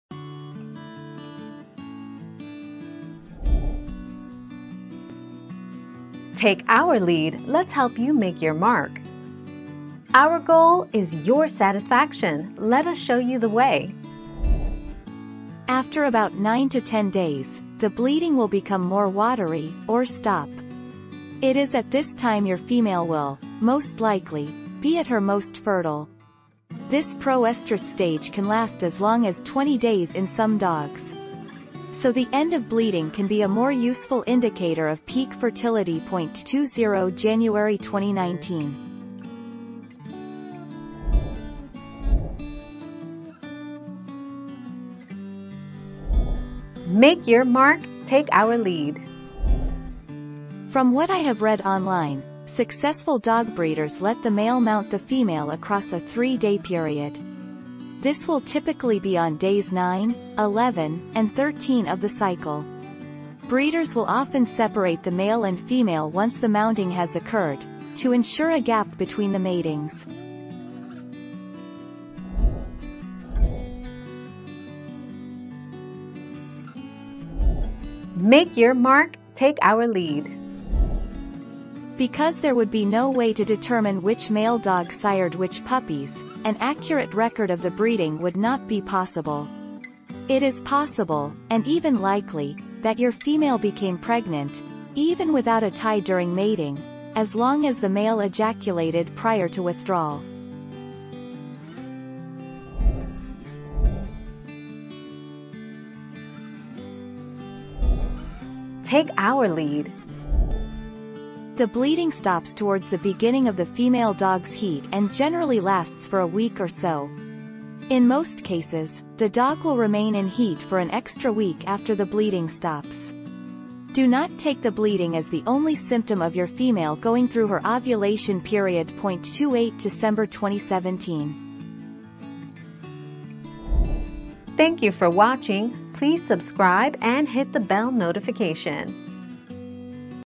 How long after bleeding is a dog fertile?
00:00 - How long after bleeding is a dog fertile?
00:43 - How many days will a female dog let a male mount her?
01:14 - Will my dog get pregnant the first time she mates?
01:44 - When my dog stops bleeding is she out of heat?

Laura S. Harris (2021, March 19.) How long after bleeding is a dog fertile?
    AskAbout.video/articles/How-long-after-bleeding-is-a-dog-fertile-230682